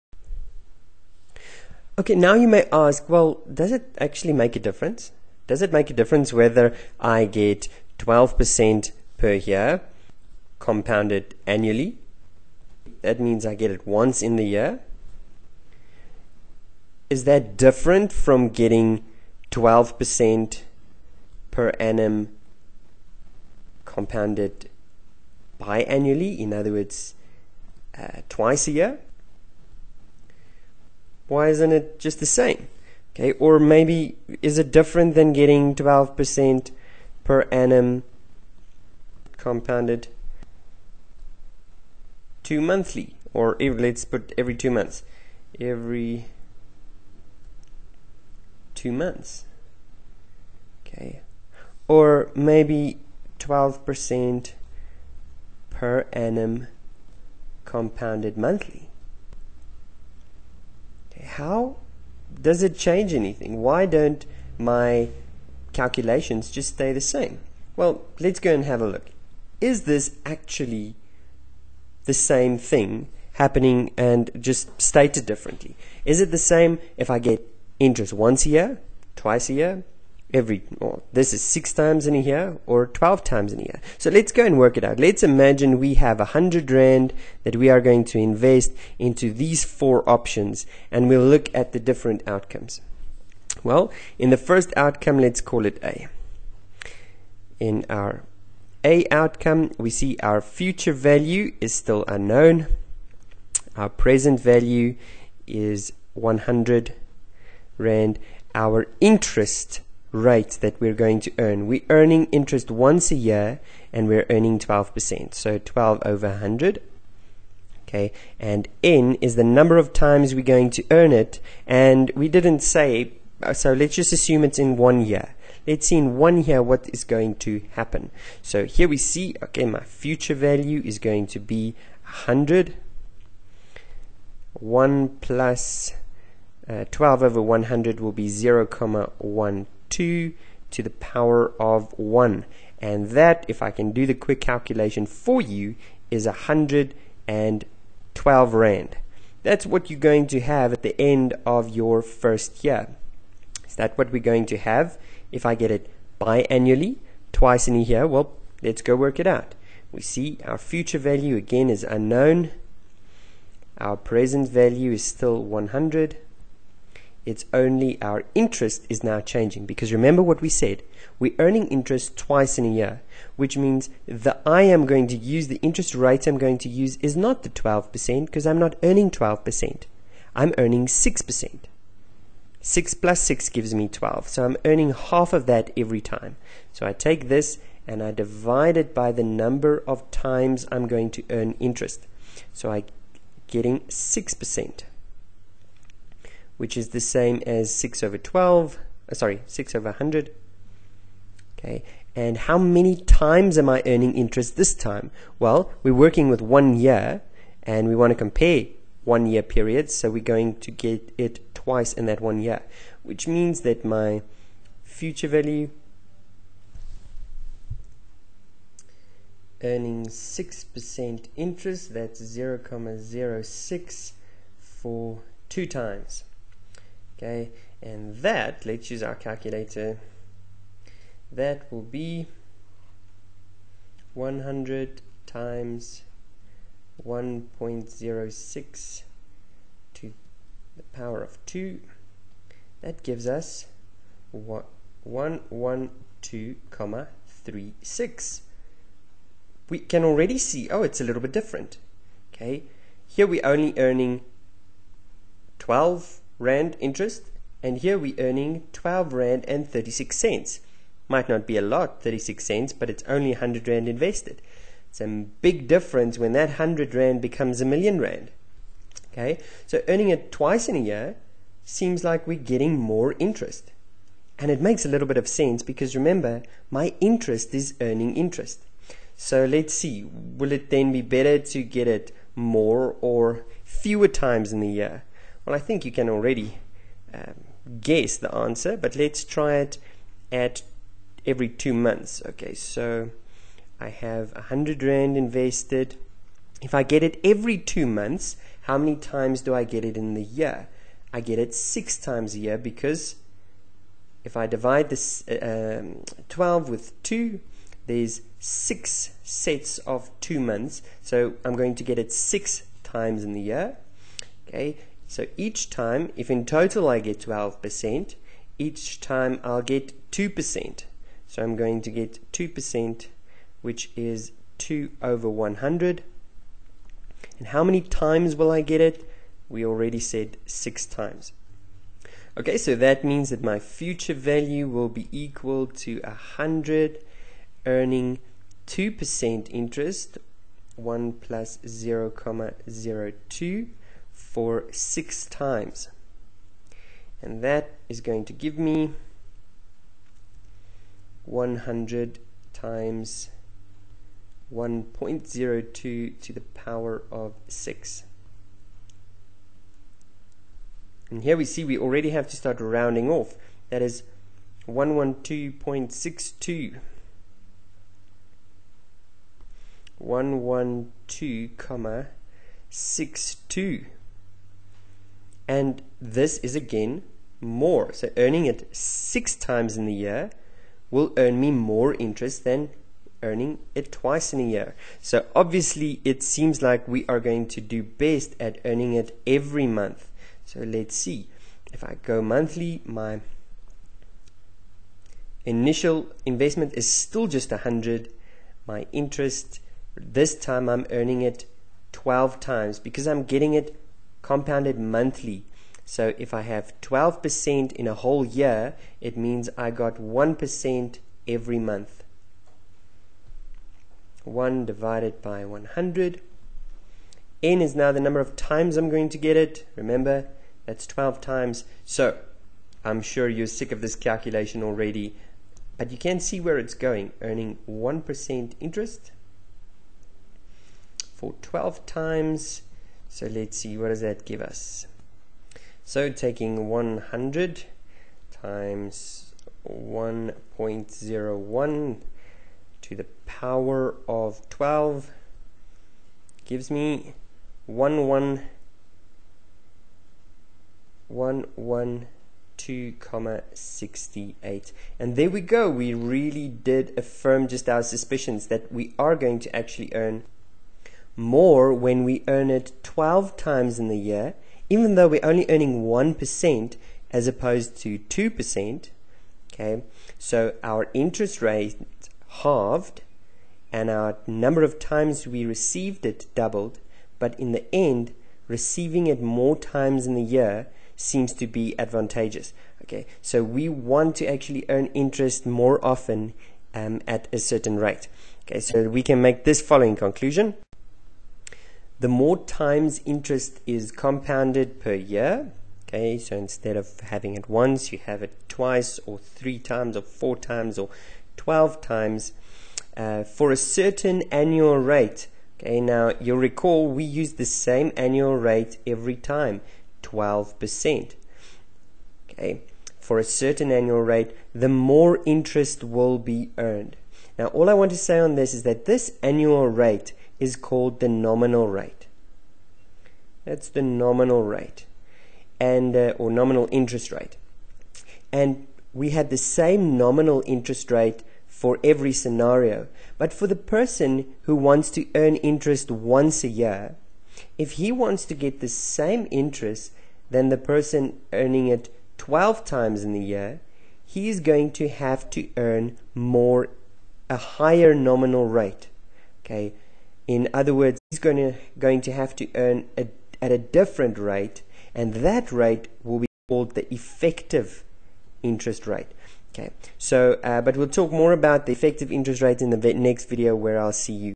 Comparing different compounding periods
Looking at a comparison of different periods of compound interest - is it better to earn interest more times in a year?